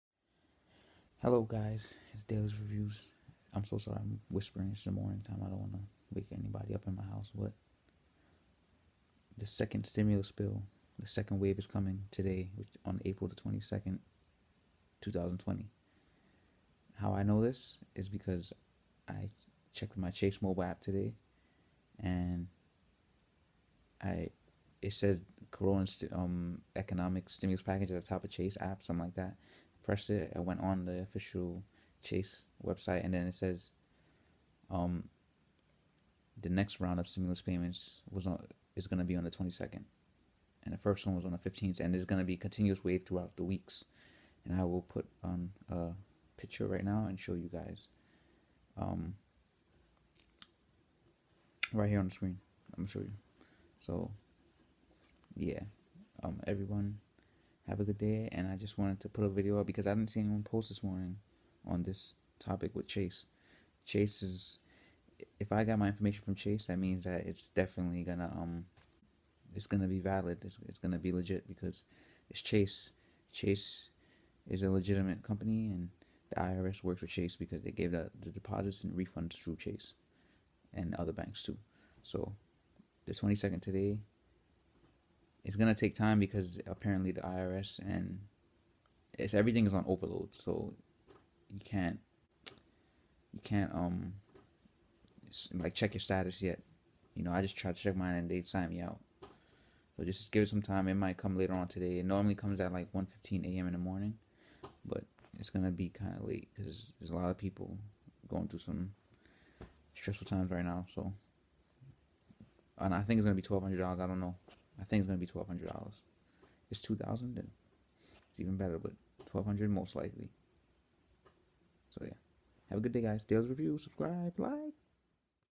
$2000 Stimulus Checks Round 2 Going To Release In May?
I’m Reading What Chase Has Notified Me About I Personally Didn’t Get a Second Check In MY Account Yet But I’ll be Checking Every 5 minutes. Chase Also Specified That They’re will be more checks coming through the coming weeks.